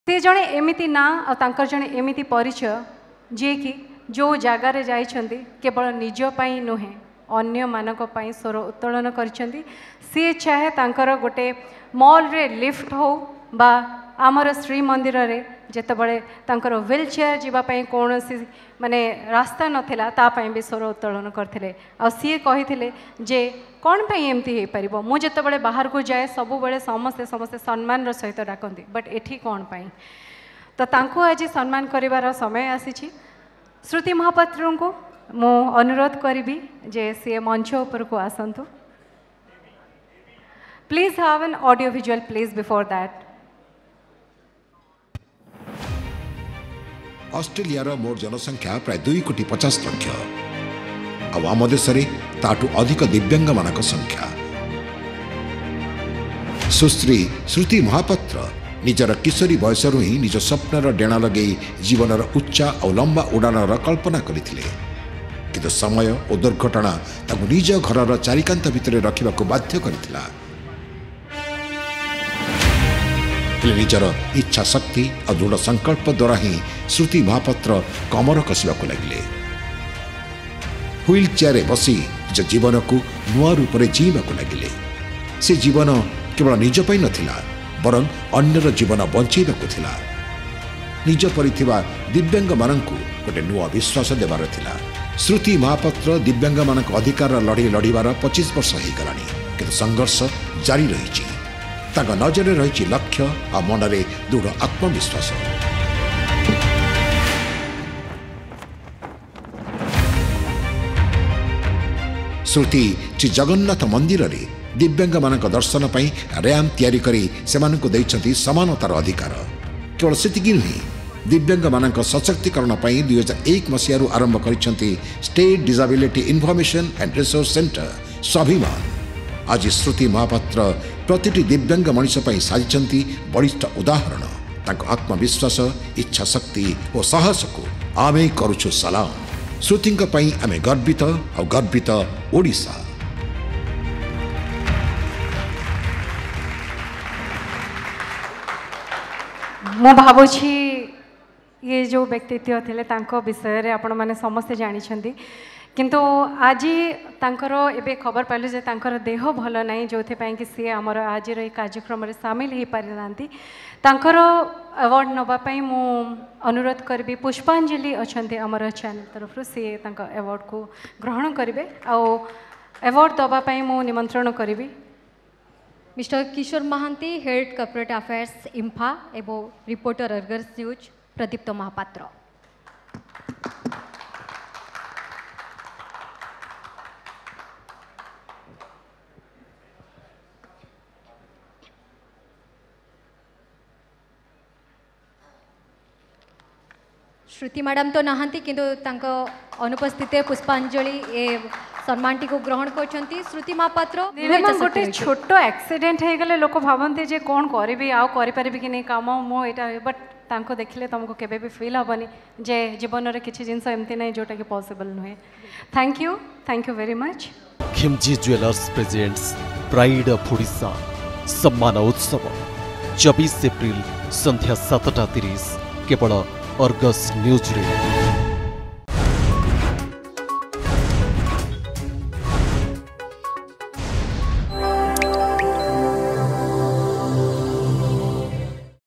Dr Sruti Mohapatra, Inspiring Dibyang Woman Who Fights for the Rights of Disabled People
Dr Sruti Mohapatra, Inspiring Dibyang Woman Who Fights for the Rights of Disabled People Felicitated at the Argus News Pride of Odisha Samman Utsav Event.